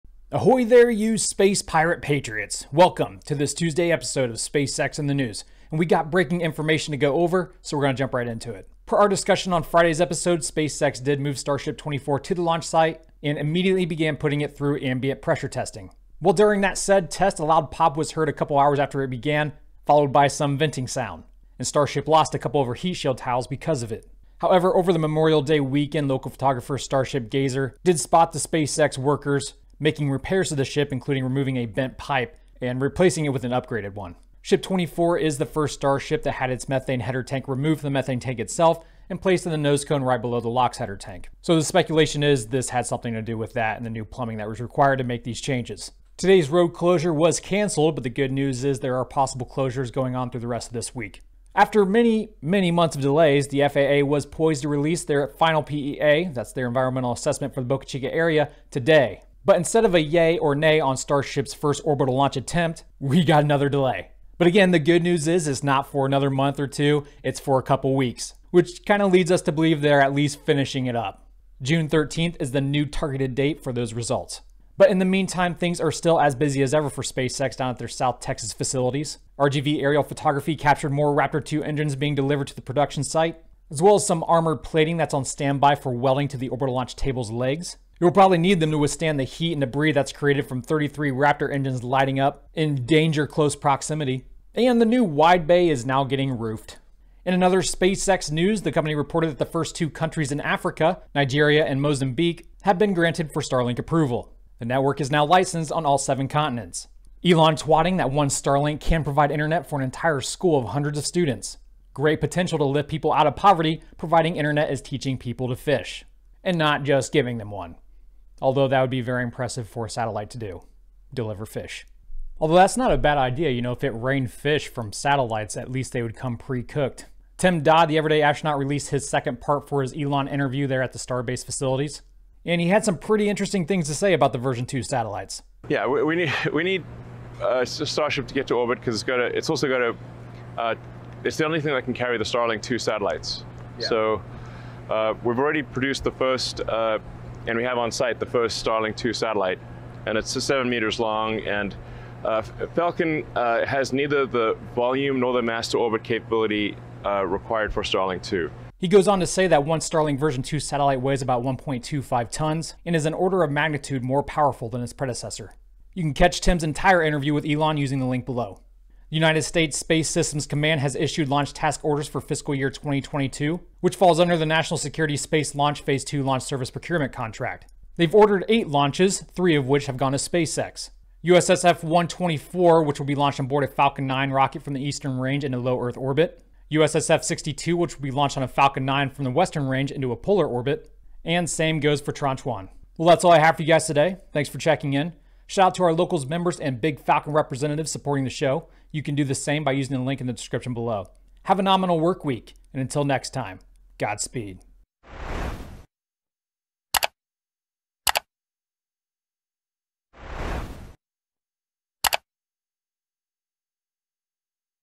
SpaceX Starship 24 Fails Test, FAA Results Scrubbed
Starship 24 encounters issues during ambient testing. The FAA delays the release of the PEA again. Good news for Starlink. And SpaceX wins more military contracts. Ep 323

TIMESTAMPS:
Intro: 0:00
Starship: 0:11
Starlink: 1:37
Falcon: 2:49